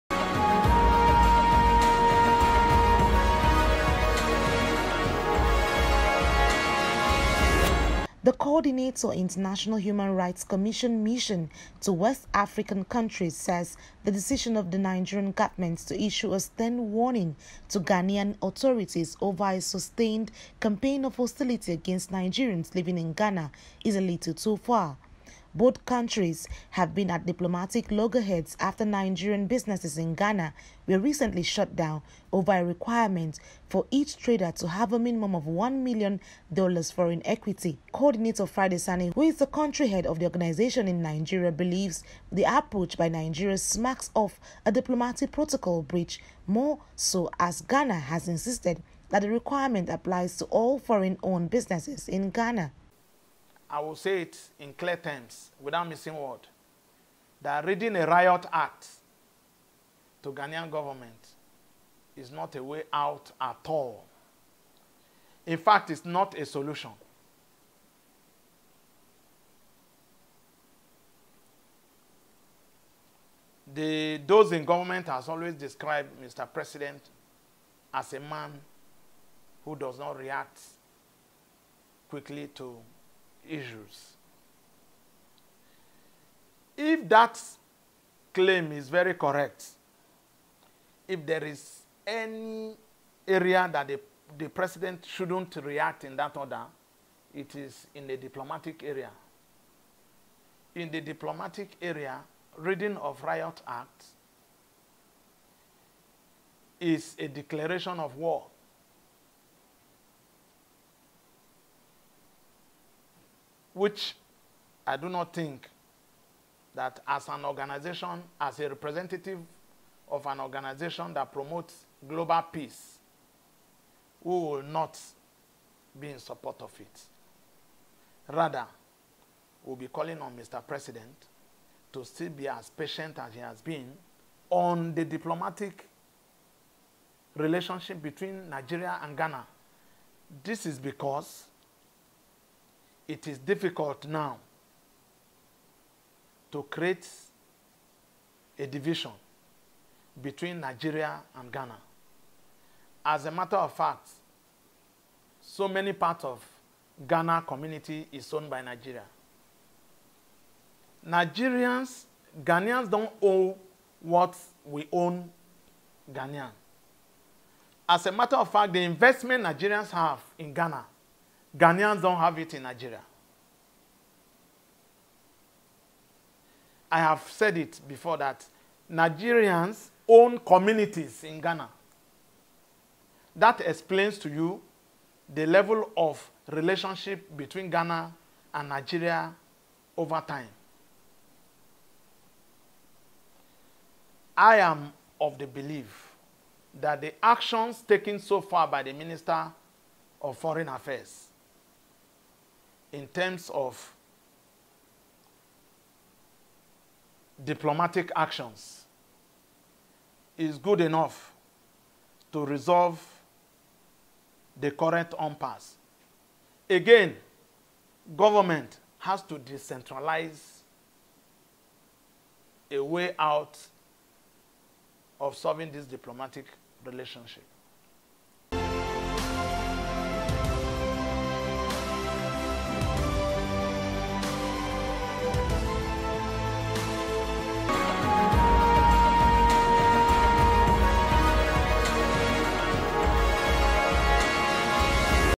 REVEALED! Diplomat Faults Nigeria's Approach To Conflict With Ghana
Diplomatic loggerhead